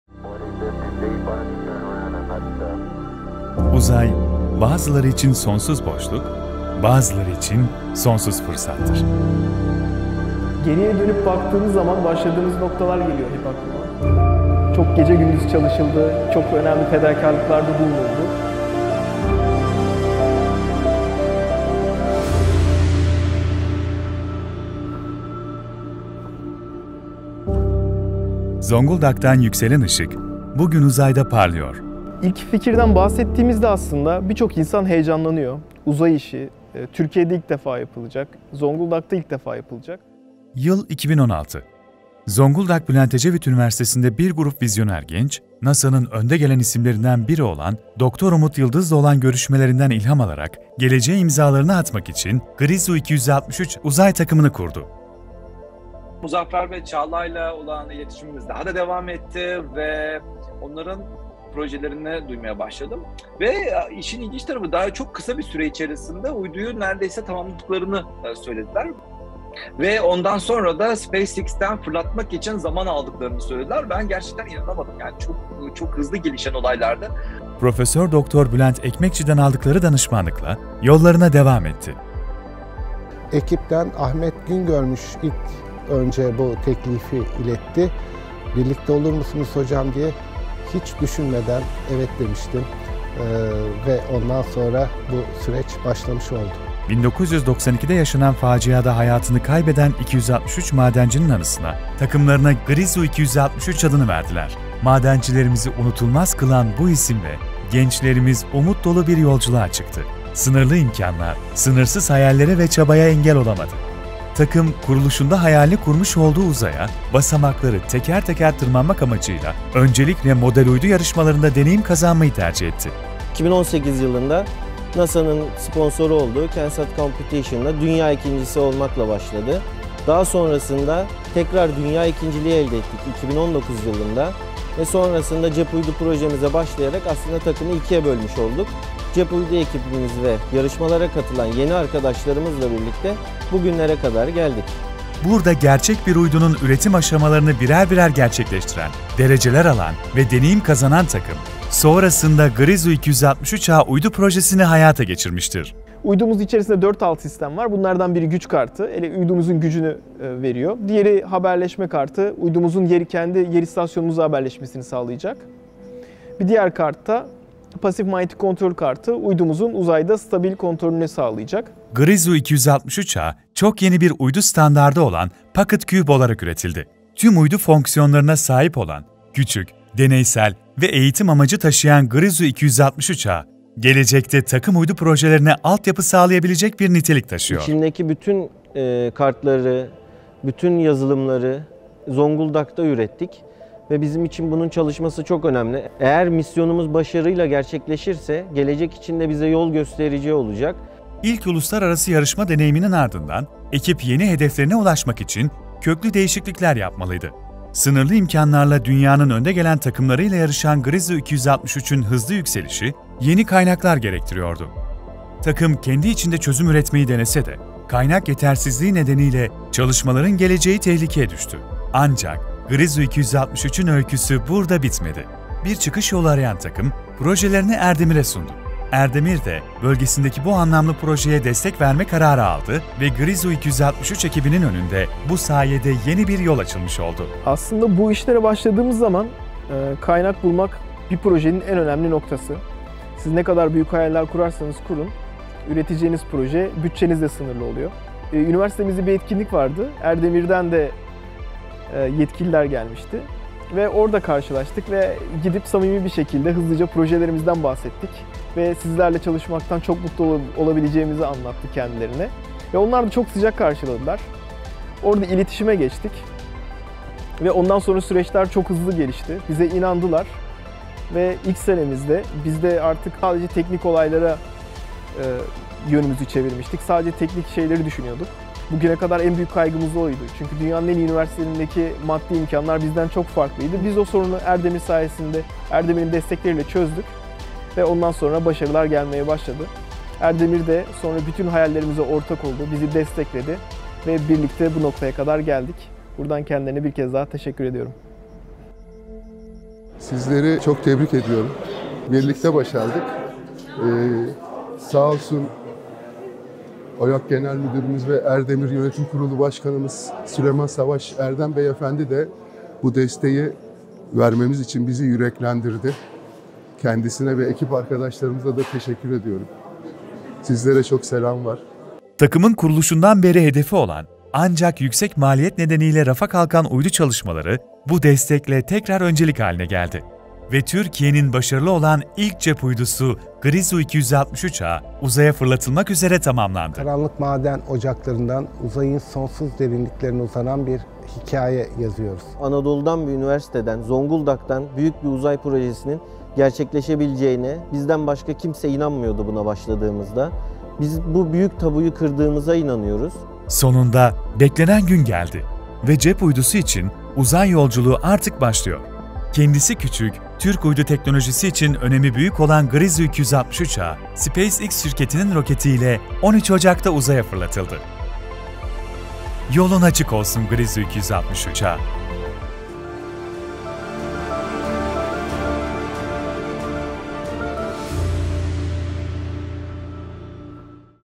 Grizu - 263: Bir Uzay Hikayesi
Zonguldak’tan, dünya uzay teknolojileri sahasına açılan Grizu-263 Uzay Takımı ile çıktığımız yolculuktan gururluyuz. Erdemir olarak desteklediğimiz Zonguldak Bülent Ecevit Üniversitesi uzay takımı Grizu-263’ün tasarladığı Grizu-263A Cep Uydusu, 13 Ocak’ta SpaceX Falcon 9 roketiyle gökyüzüyle buluştu. Yörüngeye yerleştiği ilk 10 günden bu yana dünyanın dört bir yanındaki istasyonlara binlerce veri gönderildi. Takım, uydu ile veri yansıtma sistemini ise ilerleyen günlerde test edecek.

Uzay teknolojileri sahasına ait olan Grizu-263A cep uydusu telekomünikasyon, lojistik ve pek çok alanda fayda sağlayacak ve yeni projelere ilham olacak.

Ülkemiz için değerli bir adım olan Grizu-263 projesiyle gençlerimize destek vermekten mutluluk duyuyoruz. Birlikte nice başarılara…